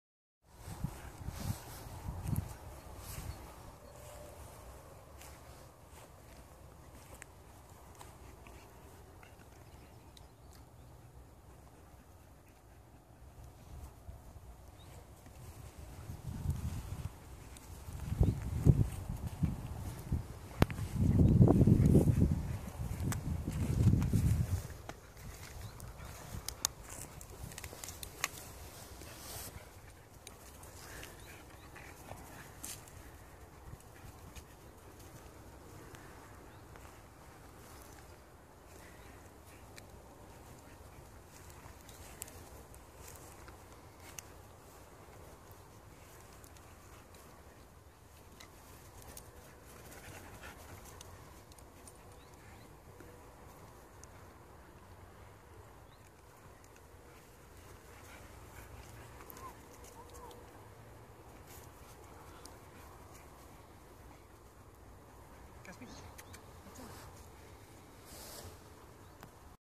2 Aussies playing tug
Best friends Casper and Wapik playing tug with a stick. Wapik loves to tease Casper like that.